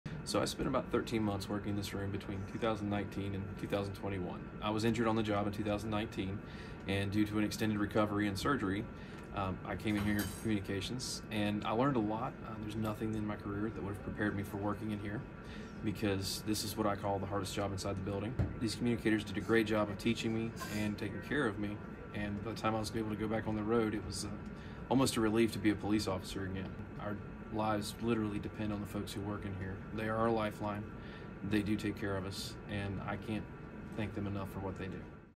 Thank You Telecommunicators | Officer Helton
We thank our telecommunicators at HPD for serving our citizens and officers.